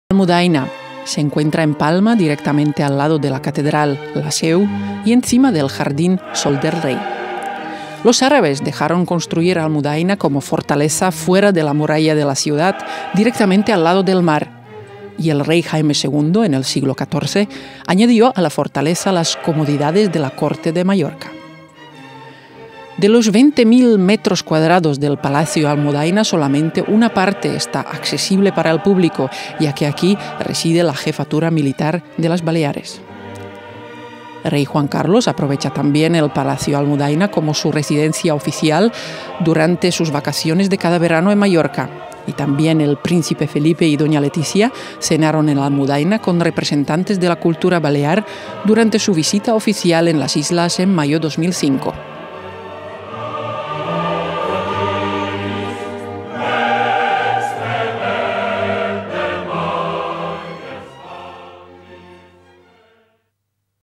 Palacio Almudaina en Palma
Palacio Almudaina se encuentra en Palma directamente al lado de la catedral La Seu y encima del jardín S´Hort del Rei. Los árabes dejaron construir Almudaina como fortaleza fuera de la muralla de la ciudad, directamente al lado del mar y el rey Jaime II en el siglo 14. añadió a la fortaleza las comodidades de la corte de Mallorca.

De los 20.000 m2 del Palacio Almudaina solamente una parte está accesible para el público, ya que aquí reside la jefatura militar de las Baleares. Rey Juan Carlos aprovecha también el Palacio Almudaina como residencia oficial durante sus vacaciones de cada verano en Mallorca. También el Príncipe Felipe y Doña Letizia cenaron en Almudaina con representantes de la cultura balear durante su visita oficial en las Islas en mayo 2005.